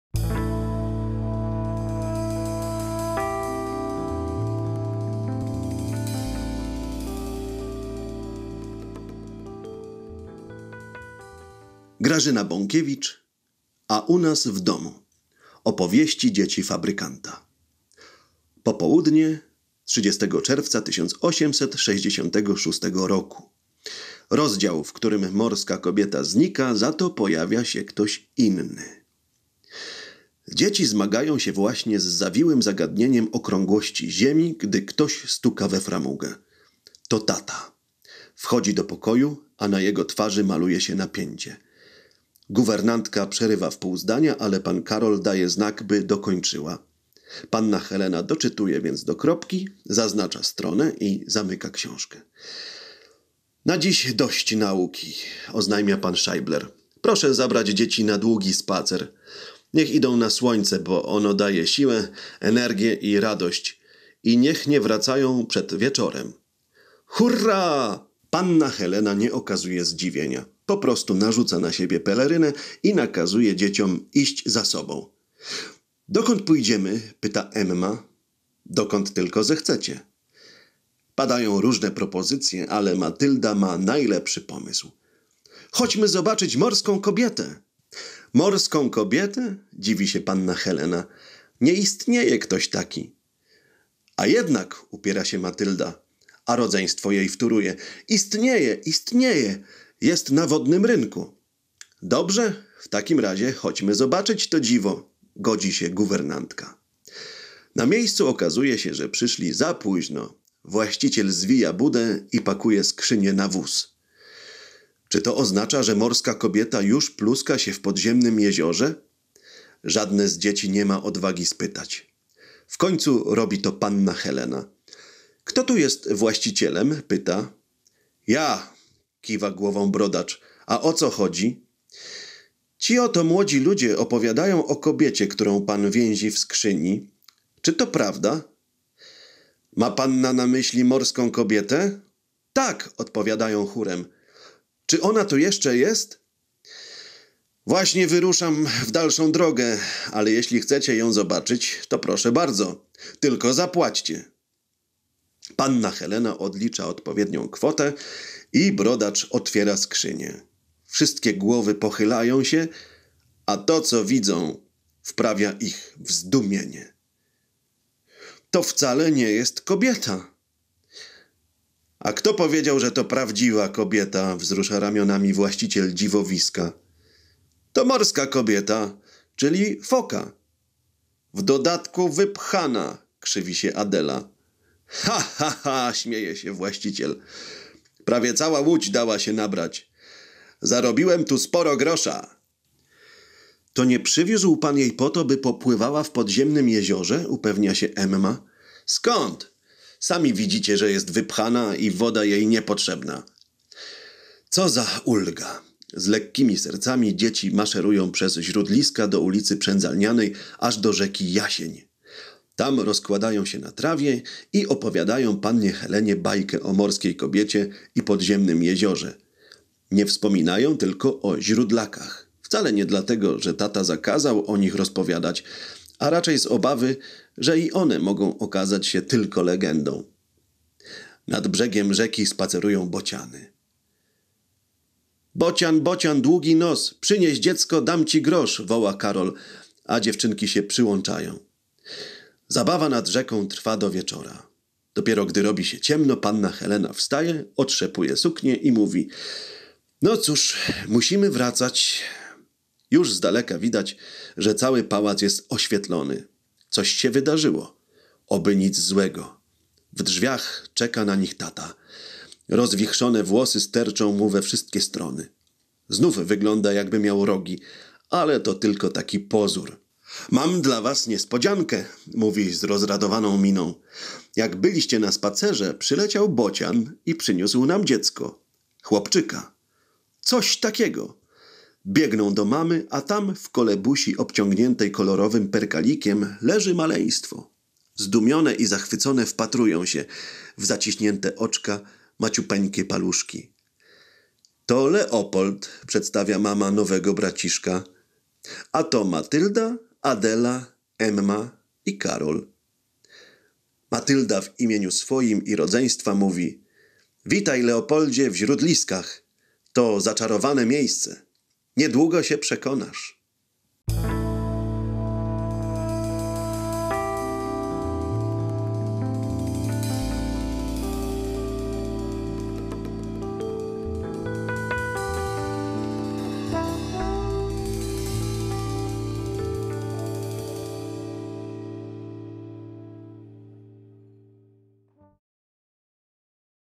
Grażyna Bąkiewicz - A u nas w domu - Opowieści dzieci fabrykanta cz. 7
Kolejna, ostatnia już część przygód dzieci Karola Scheiblera. Czyta aktor łódzkiego Teatru "Pinokio" Łukasz Bzura.